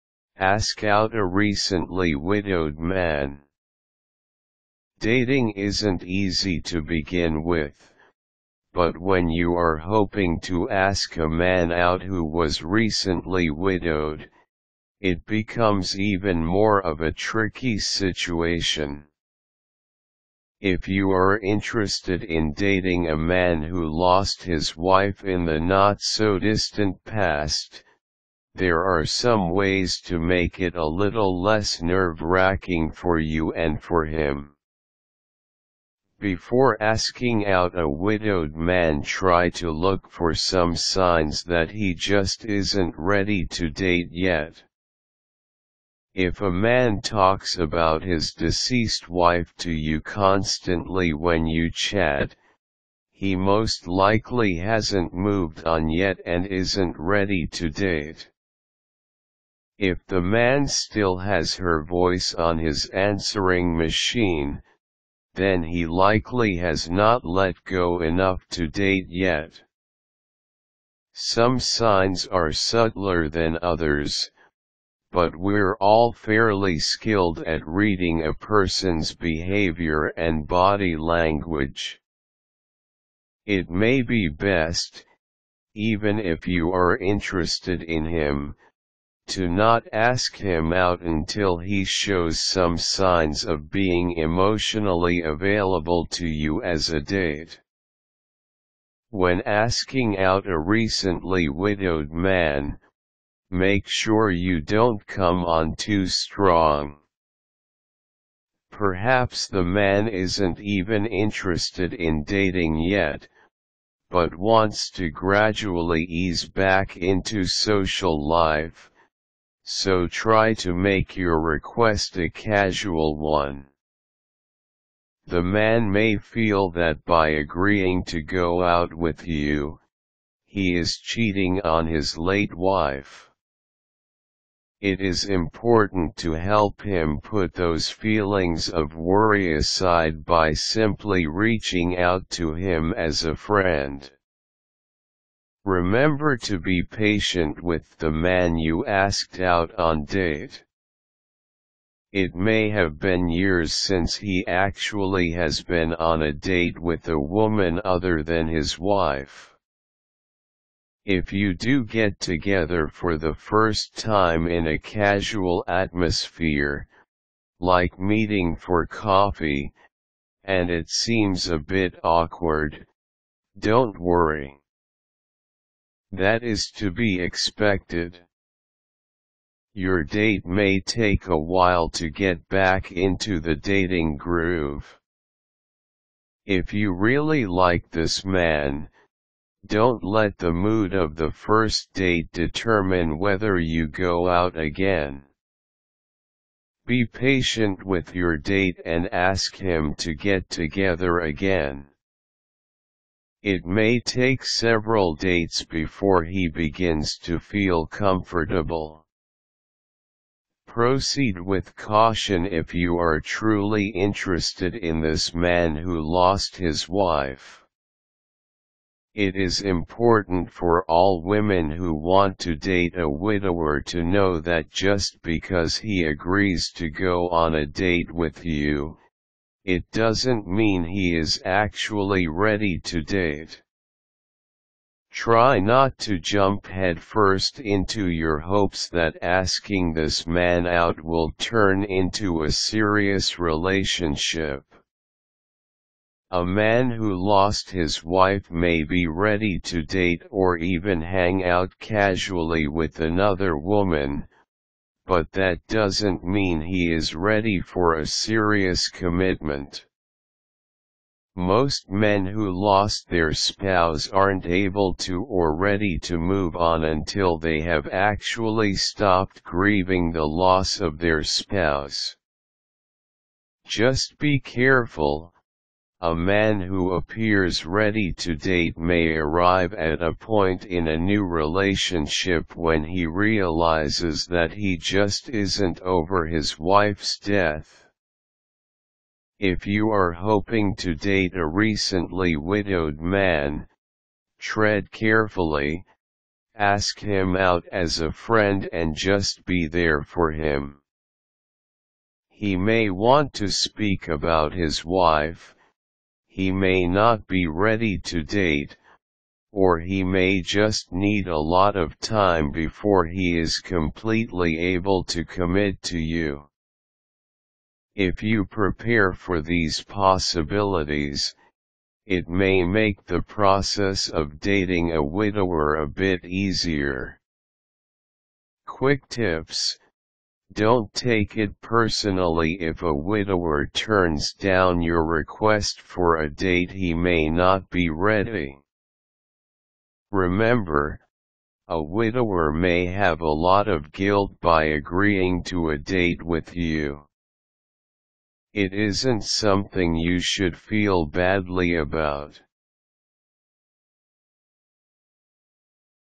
How to : Ask out a recently widowed man
Ask out a recently widowed man

 Dating isn't easy to begin with, but when you are hoping to ask a man out who was recently widowed, it becomes even more of a tricky situation. If you are interested in dating a man who lost his wife in the not-so-distant past, there are some ways to make it a little less nerve-racking for you and for him.

 Before asking out a widowed man try to look for some signs that he just isn't ready to date yet. If a man talks about his deceased wife to you constantly when you chat, he most likely hasn't moved on yet and isn't ready to date. If the man still has her voice on his answering machine, then he likely has not let go enough to date yet. Some signs are subtler than others, but we're all fairly skilled at reading a person's behavior and body language. It may be best, even if you are interested in him, to not ask him out until he shows some signs of being emotionally available to you as a date. When asking out a recently widowed man, make sure you don't come on too strong. Perhaps the man isn't even interested in dating yet, but wants to gradually ease back into social life, so try to make your request a casual one. The man may feel that by agreeing to go out with you, he is cheating on his late wife. It is important to help him put those feelings of worry aside by simply reaching out to him as a friend. Remember to be patient with the man you asked out on date. It may have been years since he actually has been on a date with a woman other than his wife. If you do get together for the first time in a casual atmosphere (like meeting for coffee) and it seems a bit awkward, don't worry. That is to be expected. Your date may take awhile to get back into the dating groove. If you really like this man, don't let the mood of the first date determine whether you go out again. Be patient with your date and ask him to get together again. It may take several dates before he begins to feel comfortable. Proceed with caution if you are truly interested in this man who lost his wife. It is important for all women who want to date a widower to know that just because he agrees to go on a date with you, it doesn't mean he is actually ready to date. Try not to jump head-first into your hopes that asking this man out will turn into a serious relationship. A man who lost his wife may be ready to date or even hang out casually with another woman, but that doesn't mean he is ready for a serious commitment. Most men who lost their spouse aren't able to or ready to move on until they have actually stopped grieving the loss of their spouse. Just be careful, a man who appears ready to date may arrive at a point in a new relationship when he realizes that he just isn't over his wife's death.

 If you are hoping to date a recently widowed man, tread carefully, ask him out as a friend and just be there for him. He may want to speak about his wife, he may not be ready to date, or he may just need a lot of time before he is completely able to commit to you. If you prepare for these possibilities, it may make the process of dating a widower a bit easier.

  Quick Tips: Don't take it personally if a widower turns down your request for a date -- he may not be ready. Remember, a widower may have a lot of guilt by agreeing to a date with you. It isn't something you should feel badly about.